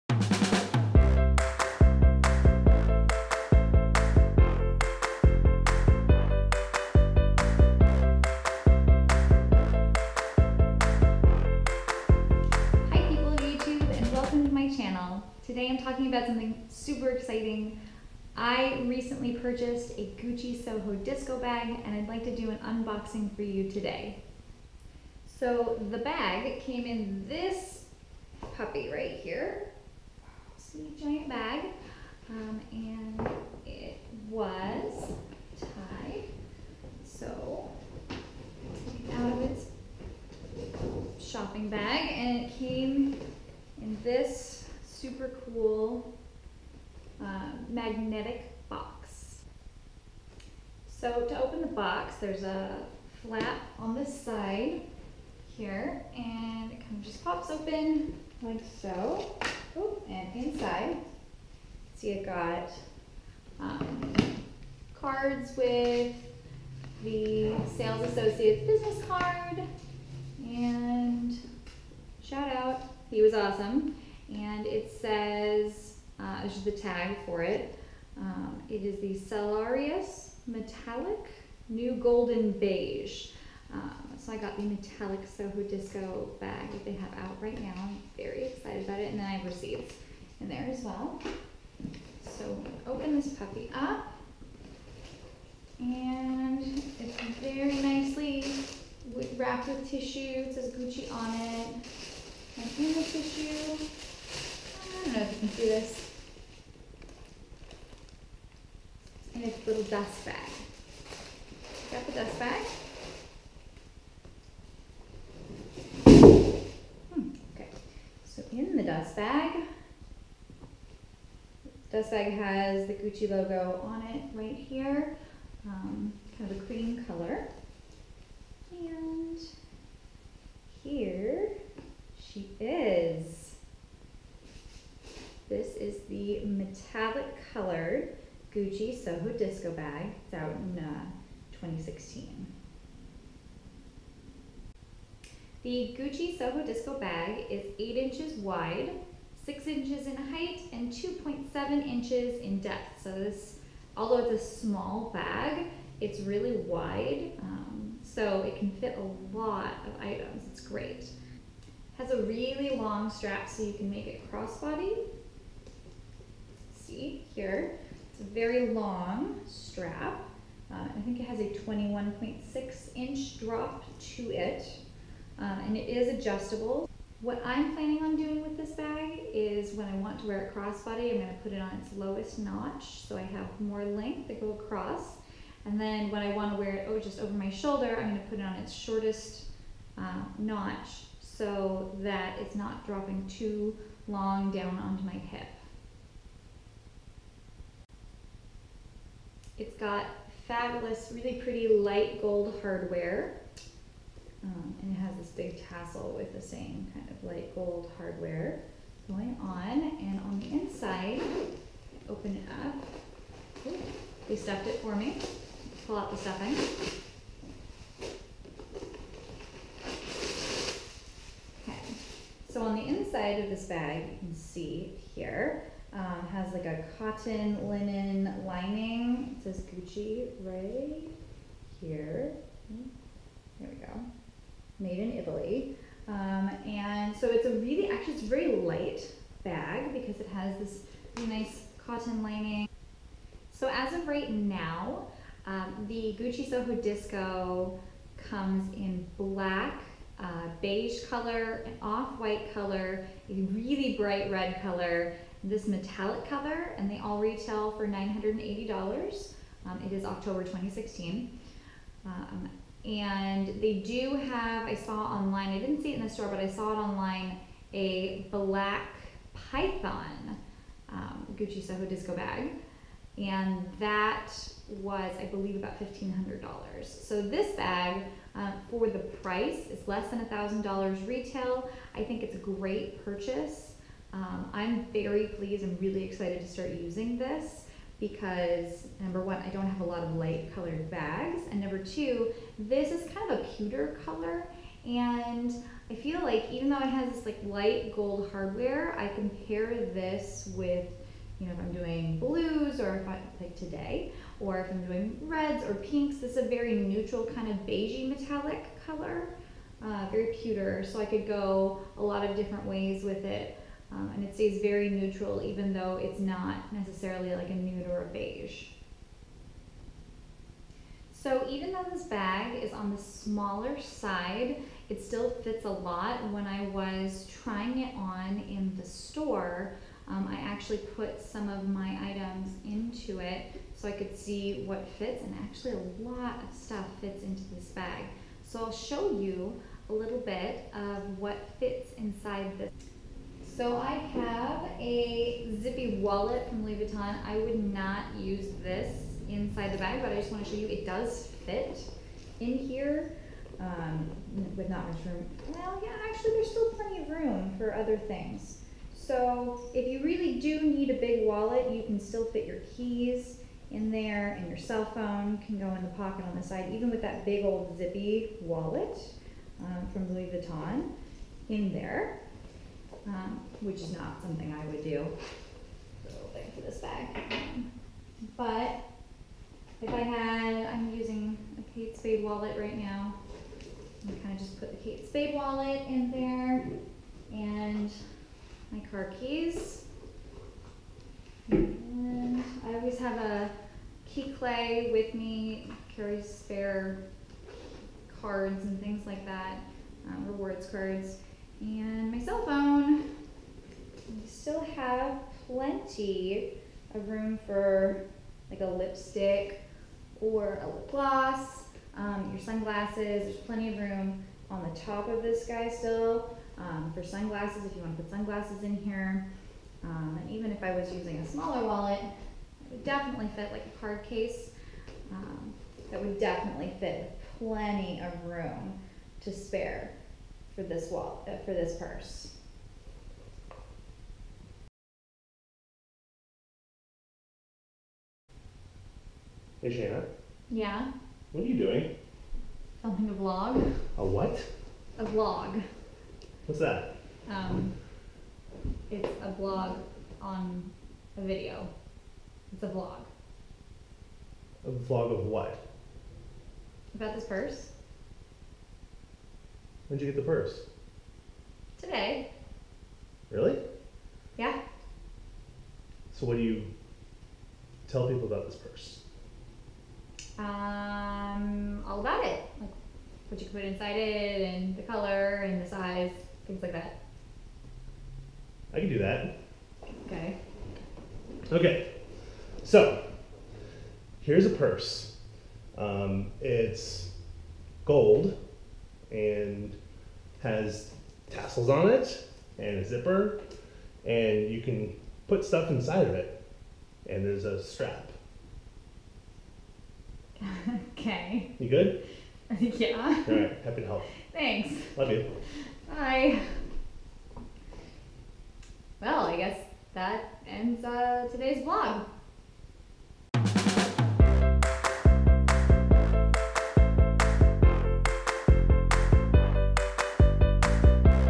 Gucci Soho Disco Bag Unboxing | Retail Therapy Chick
Gucci Soho Disco Bag Unboxing!

Things I use to film:
Super cool camera
Large tripod
Baby tripod
Lavalier mic
Fuzzy camera mic
Softbox lights
Ring light